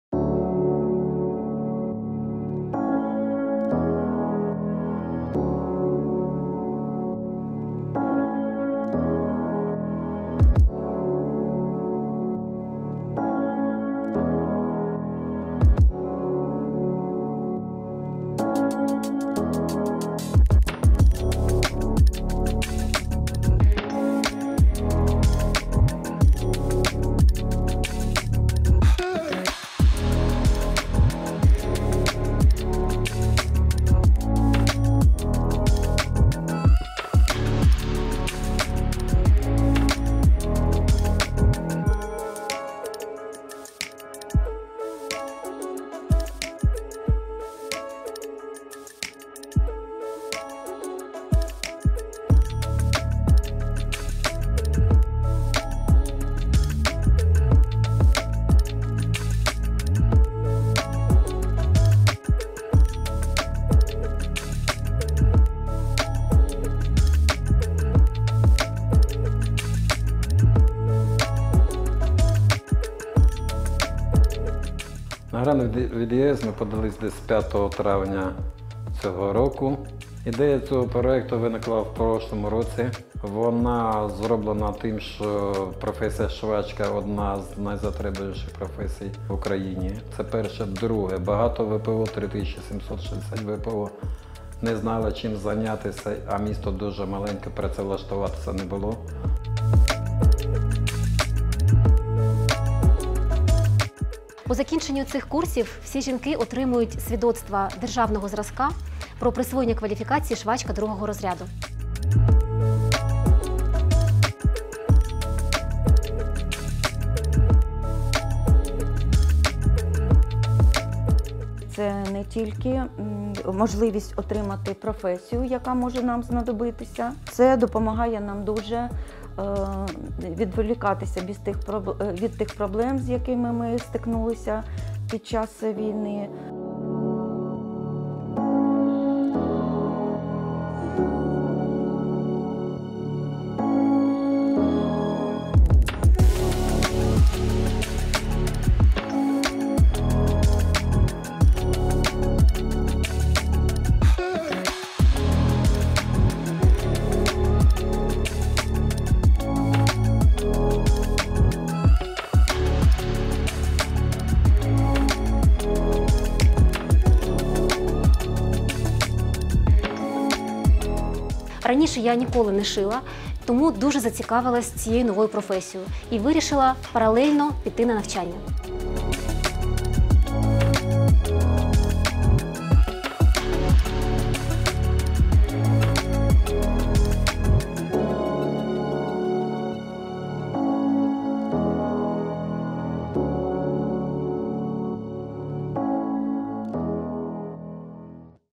Історія переселенки, що знайшла себе у швейній справі
Ця історія про жінку, яка втекла від війни з рідного Запоріжжя і змогла почати життя спочатку за 900 км від дому — в Бережанах.

Саме там Олена Бережна змогла успішно реалізуватись у новій професії, тому нині допомагає іншим переселенкам знайти роботу завдяки співпраці з ГО «Креативний хаб Бережани».

Жінка працює керівницею проєкту «Школа-ательє Бережани», який діє у місті за фінансової підтримки ЄС.

Як переселенки з різних куточків країни завдяки цьому стають на ноги, дивіться в новому ролику ШоТам!